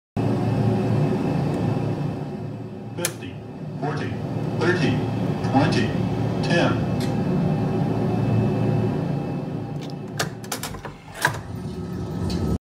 CFY throttle: Autolanding with Zibo B737 ver. 3.49.11
When Autolanding with Zibo B737 ver. 3.49.11, CFY throttle levers moves.
But throttle levers stops before the idle.